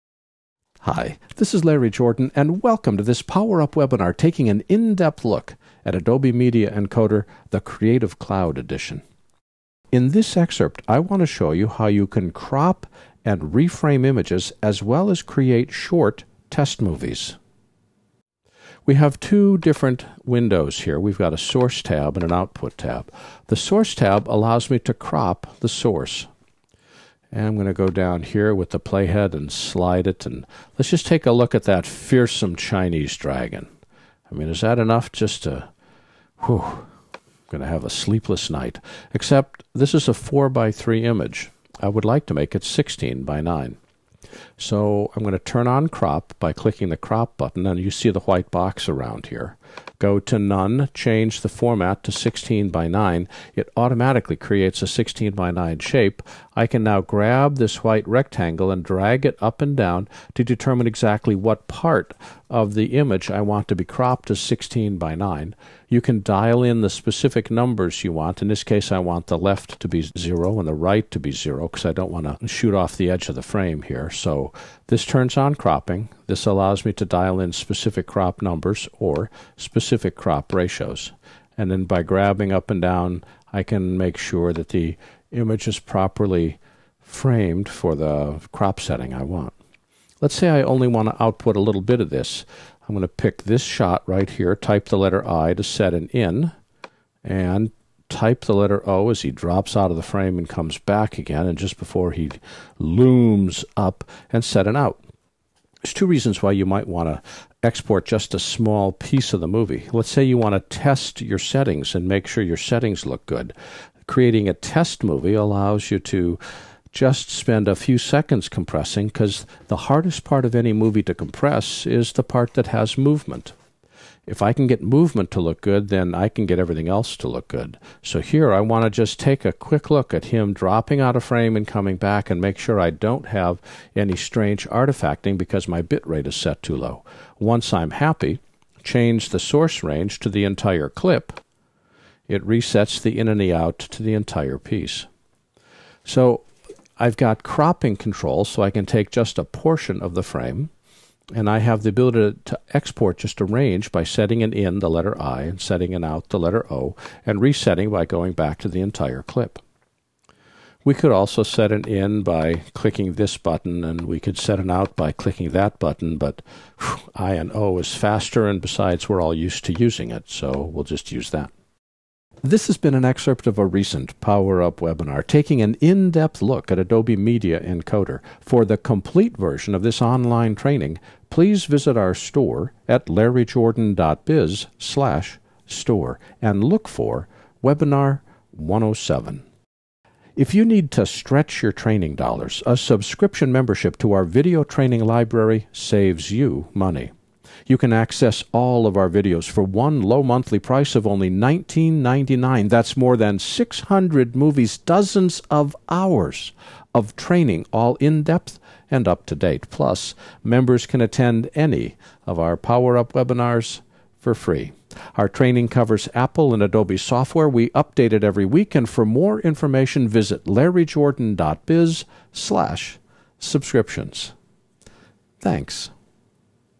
Crop & Reframe Images in Adobe Media Encoder CC
- Looking to create quick test movies? Need to crop and reframe images? The brand new Adobe Media Encoder CC opens up new possibilities for all of these, and this excerpt from our recent "Adobe Media Encoder CC: In-Depth" webinar, shows you how! - Enjoy!